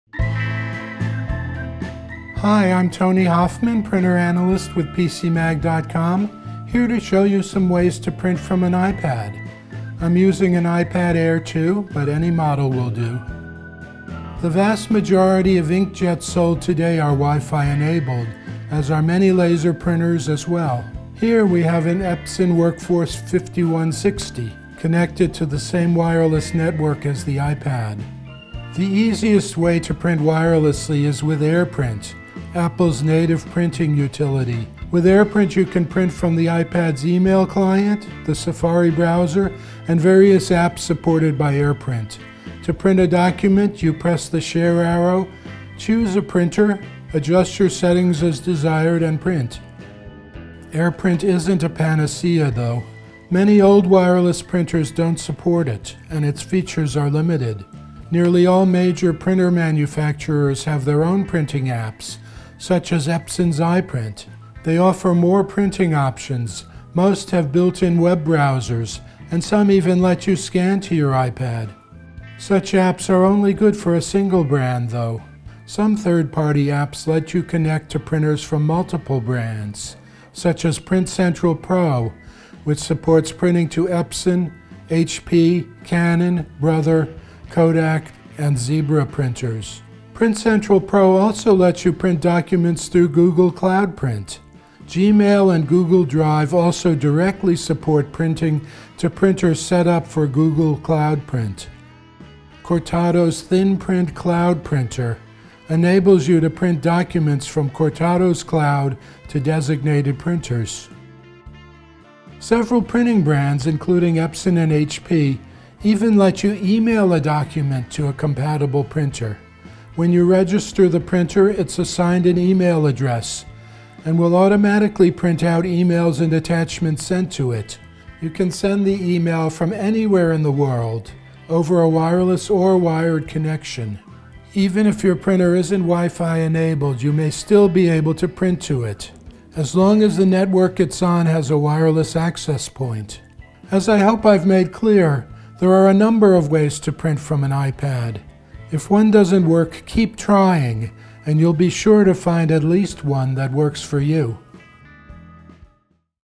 How to Print from an iPad
There are many ways to print directly from your iPad. We'll show you a few.